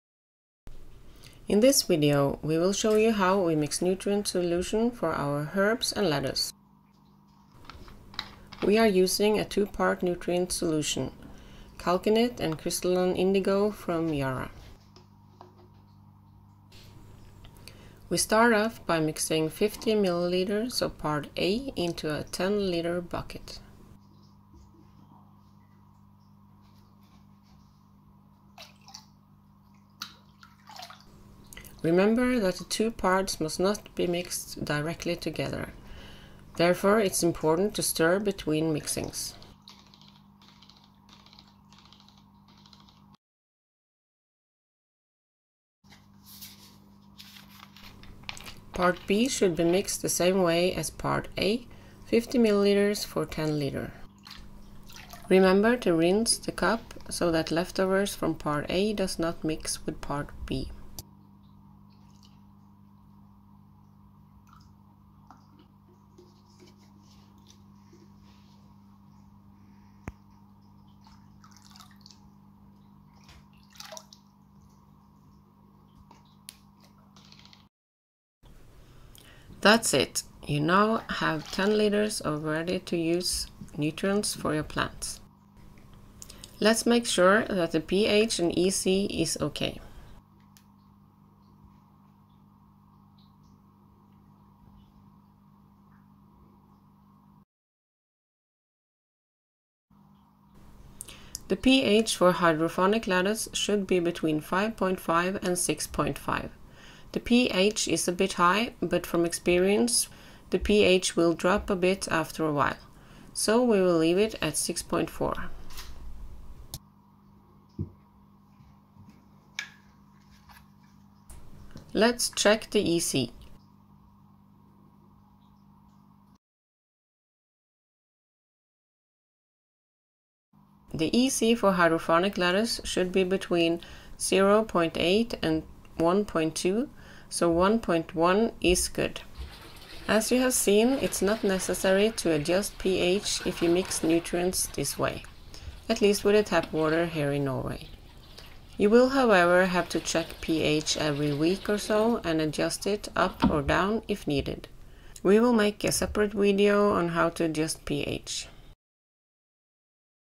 Mixing A+B Hydroponic Nutrient solution
In the video we are mixing nutrients for our lettuce and herbs in the NFT-system. We check the pH and EC. See how we mix our stock solution from powder

Kristalon Indigo and CalciNit is sold by Yara only in 25 kg sacks for professionals. We have therefore on beforehand mixed a stock solution you see in this video using 50 g powder of each with 0,5 L water and poured it on 0,5 L easy to use bottles. Each small bottle will give you 100 liters of nutrient solution (ca 1.2 EC). This gives you cheap hydroponic nutrients :-)